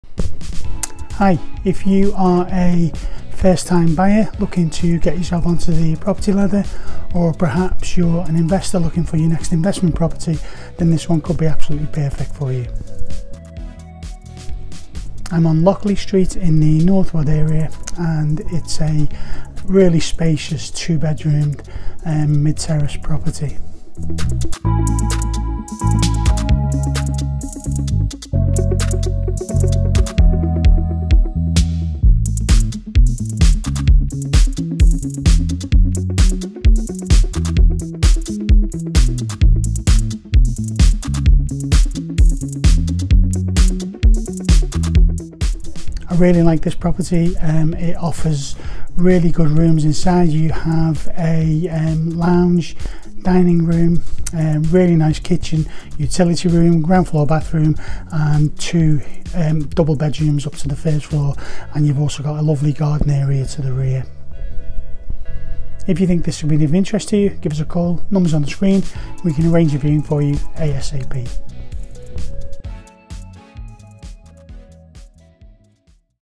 House for sale on Lockley Street, Stoke on Trent
We could all do with some extra space when it comes to our home, and this terraced property will not disappoint. A lovely home that gives you the room you need, this deceptively sized mid terraced home needs to be viewed to be appreciated. Comprising in brief, lounge, dining room, kitchen, utility room, ground floor bathroom and two double bedrooms, the property also benefits from a garden to the rear.